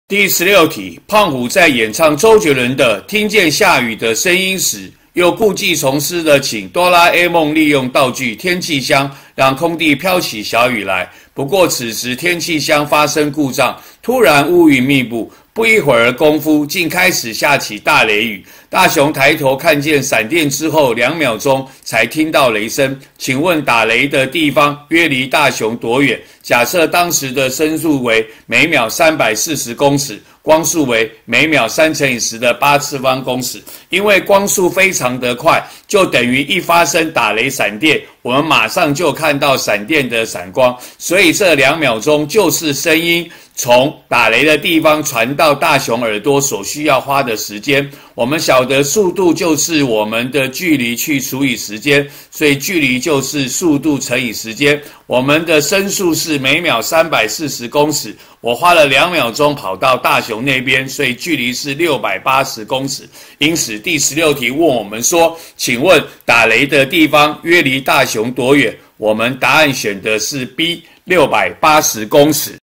108學年上8年級理化二段第16題
第5～16題題組：
大雄和多啦A夢參加胖虎在空地舉行的個人演唱會。

第16題問：看到閃電2秒聽到雷聲，問打雷處距離多遠。